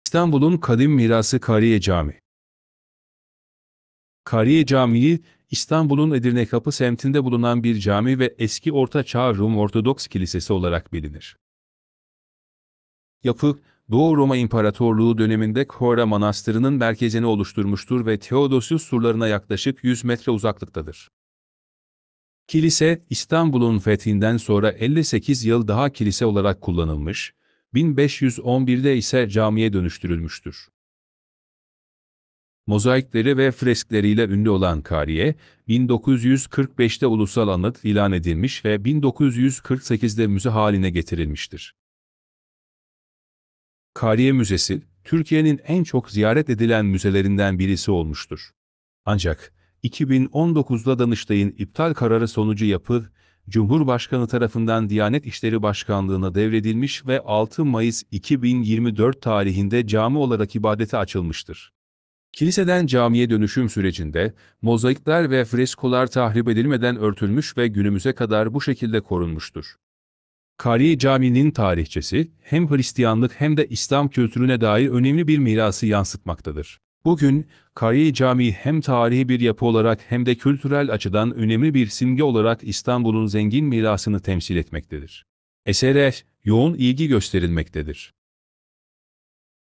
Kariye Cami'nin Kilise, Müze, Cami Yolculuğu
Kiliseden camiye çevrilen Kariye Camii'nin hikayesi.